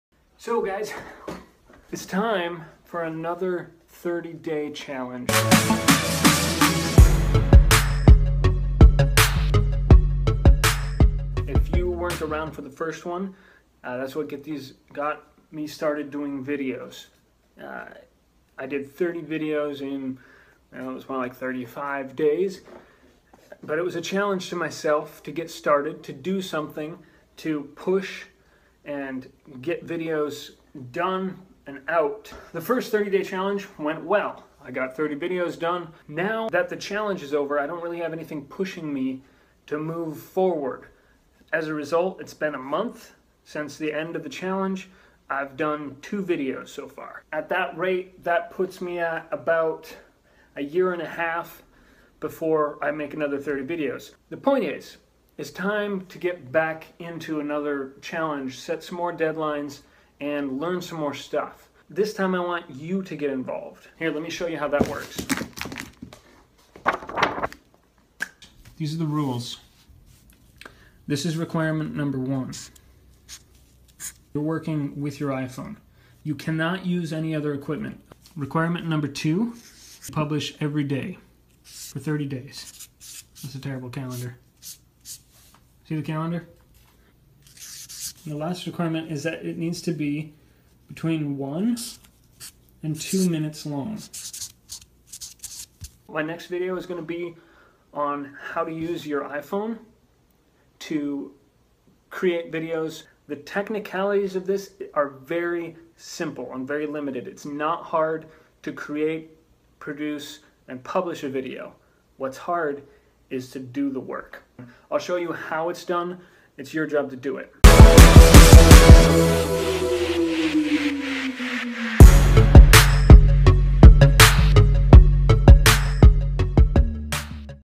I CHALLENGE YOU
I'm feeling complacent. It's time to put on some steam.

JOIN ME - SEE INSTRUCTIONS BELOW

30 DAY CHALLENGE RULES
1. Phones only - no other gear!
2. Publish every day for 30 days
3. Videos should be 1-2 minutes long

I'm super excited for this! I want you to join me and push yourself to create daily. This challenge will definitely help you to get better at film-making and videos in general, but most importantly it will get you in the habit of trying. For me, this has been the most difficult part - getting over my self-doubt and high expectations and just getting it done.

Send me a Facebook or Instagram message saying "I'm in!". I'll get you started, and add you to the private Facebook group where you can meet other 30Day Crew members (is that a name?) and share questions, tips, resources and ideas with the crew.